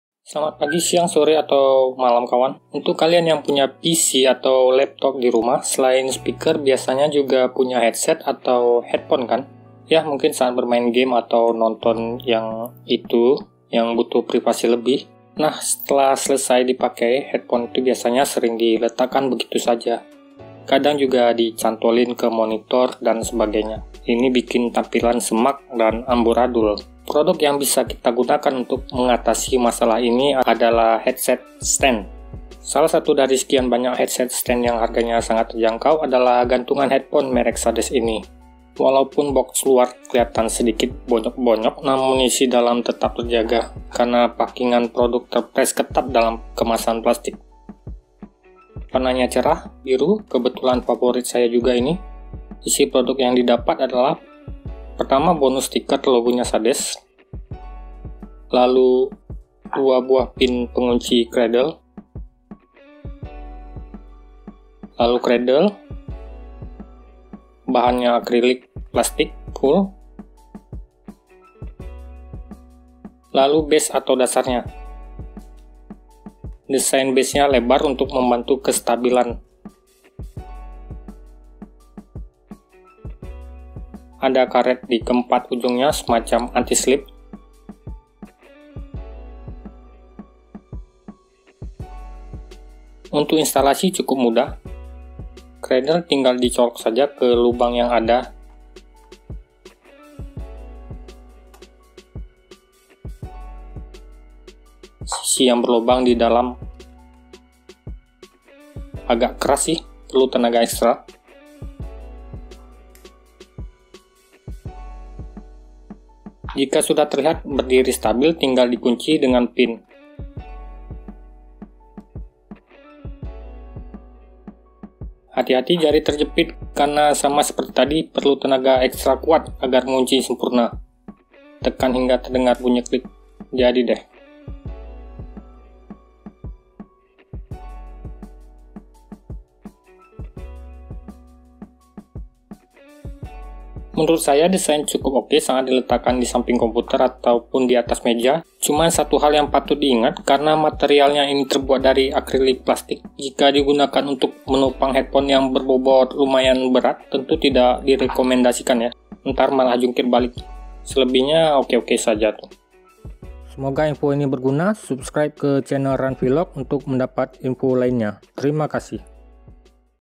Headset Stand/Gantungan headphone Murah
Gantungan headphone buatan sades dengan tipe Wolfbone ini bukanlah barang baru dipasaran. Namun masih oke untuk dijadikan pilihan, ketimbang produk sejenis (yang berharga lebih mahal dan fungsi yang sama).

Ukuran
Tinggi: 26.5 cm
Panjang: 16.5 cm
lebar: 13.2 cm
Varian warna: Biru & Merah.